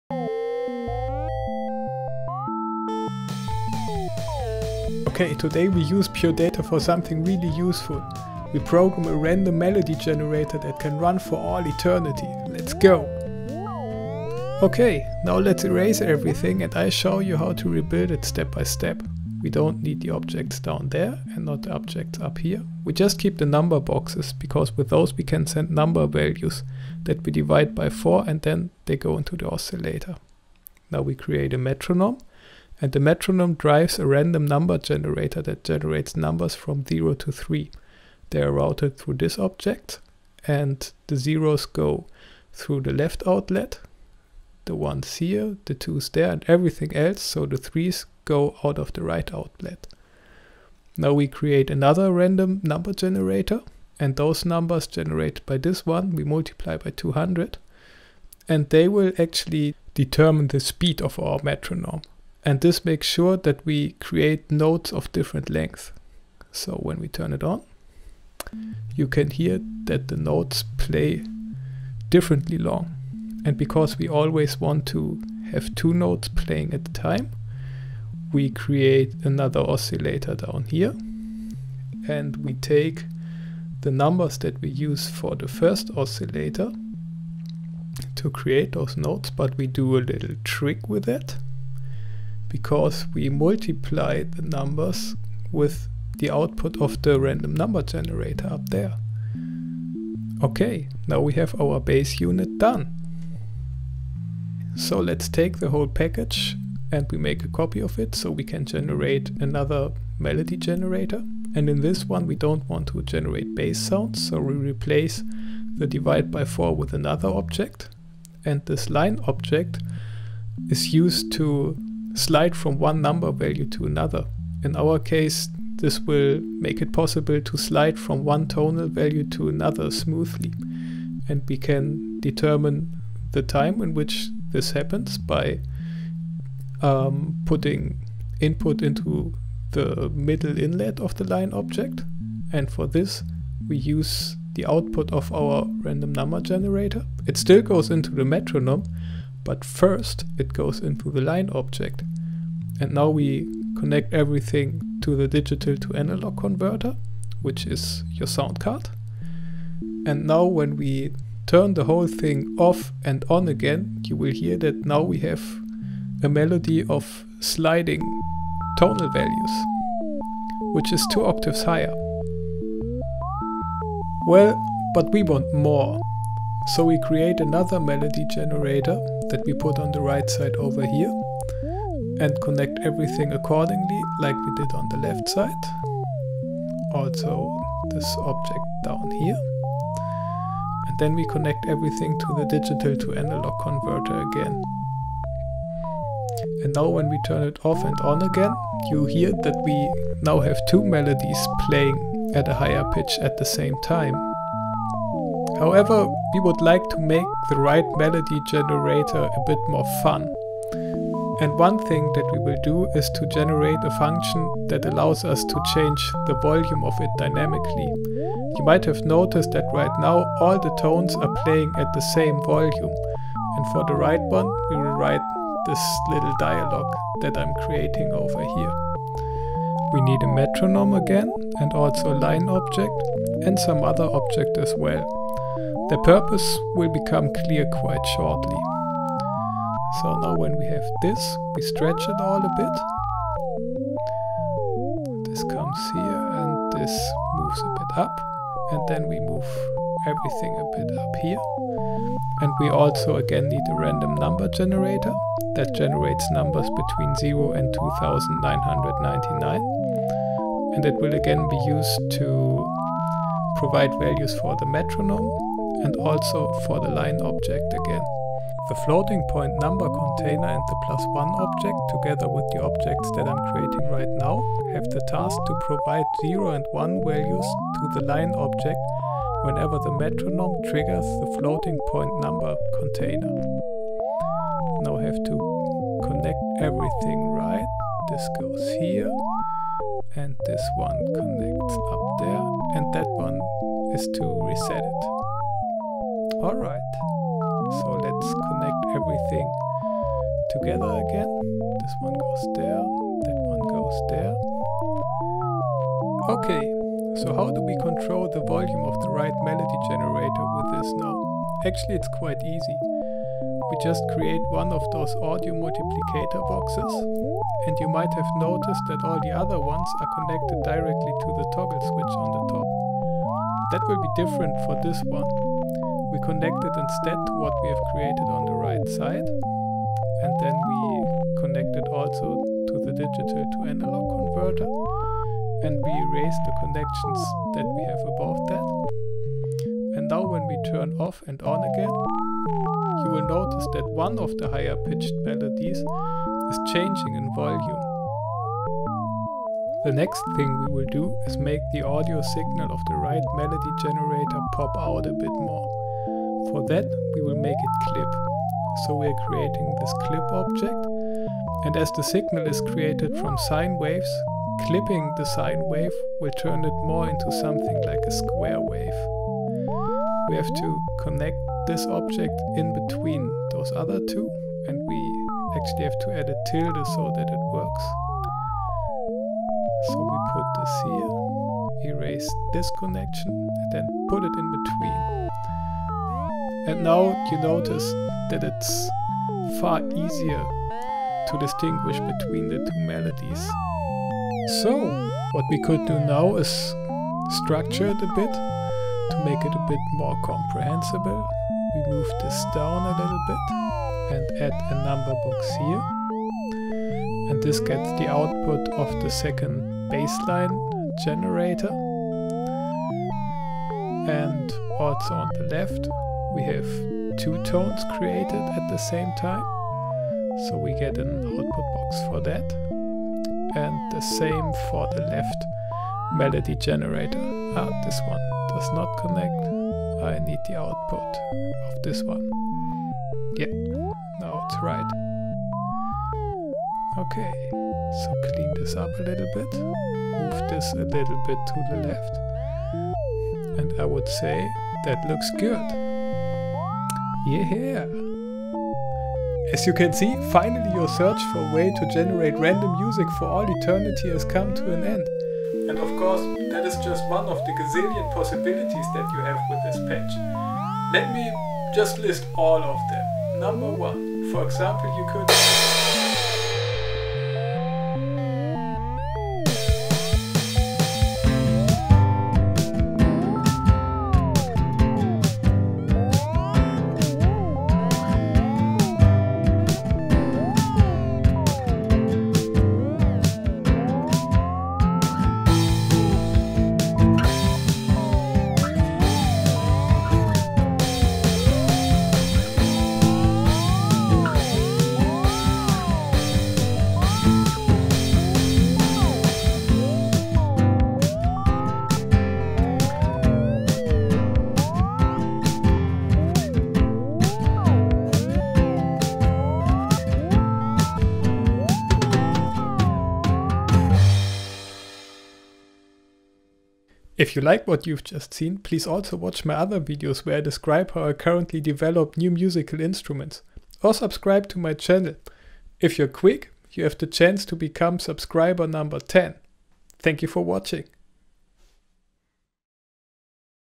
Random Melody Generator RUNS FOREVER! pure data realtime programming tutorial
In this video I will show step by step how I program a random music generator in pure data vanilla and jam to it in the end!